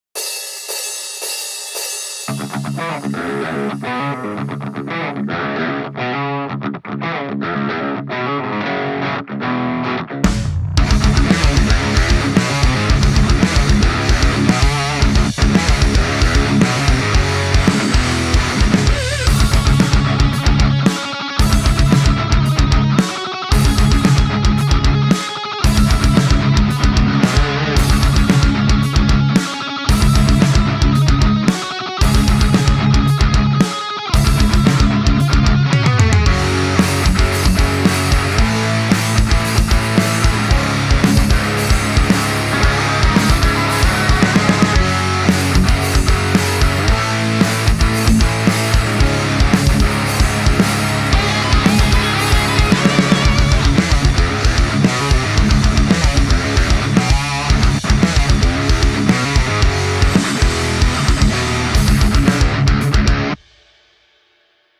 Boss Katana 50 Metal Test
Gear:

Chapman Ghost Fret (2015)
Seymour Duncan distortion (sh-6) bridge / 59 neck
Boss Katana 50 combo amplifier
Focusrite 2i2 Interface
Reaper DAW